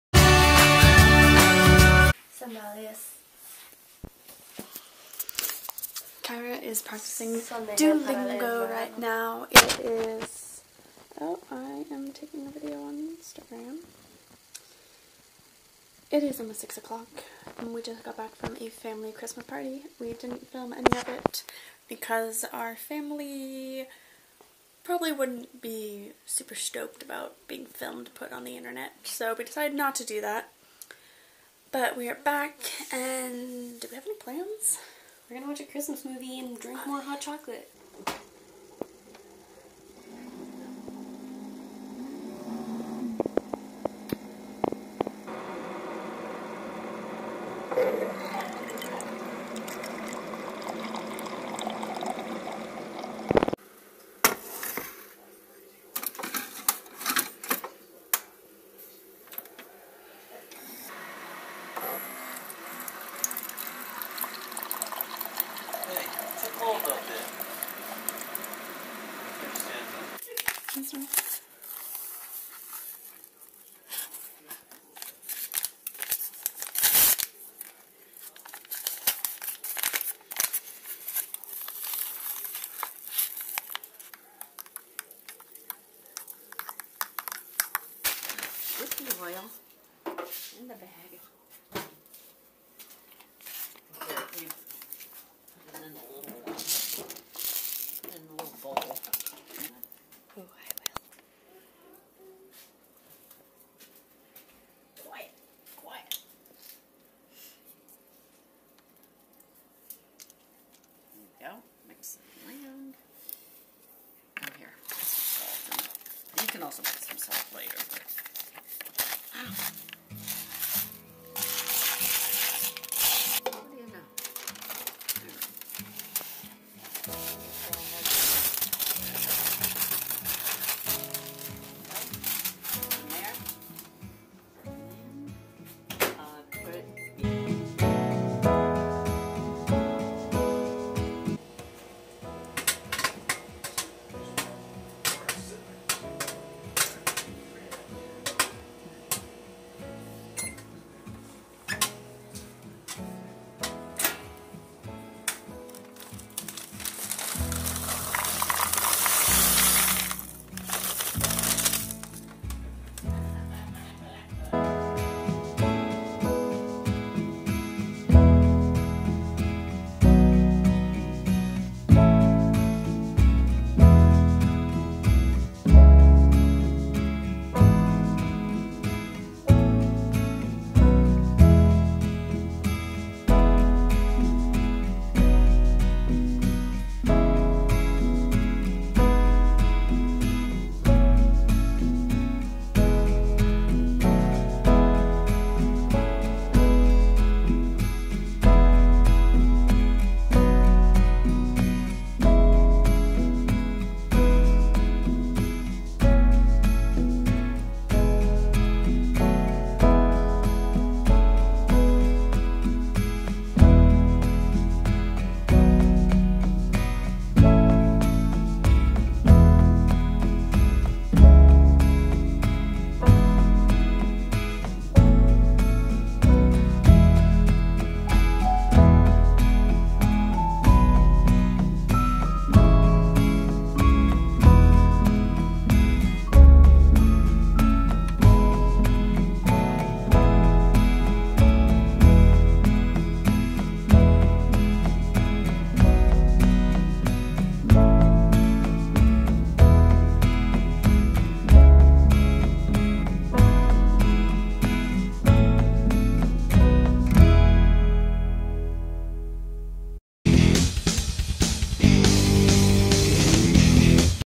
WE'RE ARTISTS!!! | VLOGMAS DAY 23
we do a lot of things. but we didn't film any of it :(

WE did however make hot chocolate watch a few christmas movies make popcorn and do some crafty crafts!!

Peace and Blessingz,

The Twin Thing